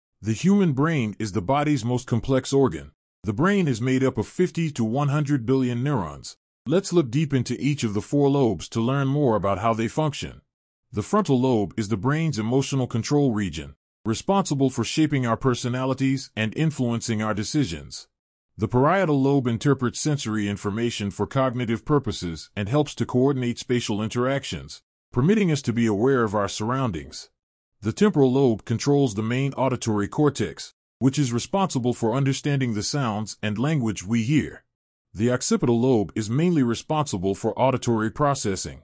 The Human Brain Explained (Animation)
The human brain is the body's most complex organ. The brain is made up of 50 to 100 billion neurons, let's look deep into each of the four lobes to learn more about how they function. The frontal lobe is the brain's emotional control region, responsible for shaping our personalities and influencing our decisions. The parietal lobe interprets sensory information for cognitive purposes and helps to coordinate spatial interactions, permitting us to be aware of our surroundings. The temporal lobe controls the main auditory cortex, which is responsible for understanding the sounds and language we hear. The occipital lobe is mainly responsible for auditory processing.

The information and resources provided on this channel are for educational and informational purposes only.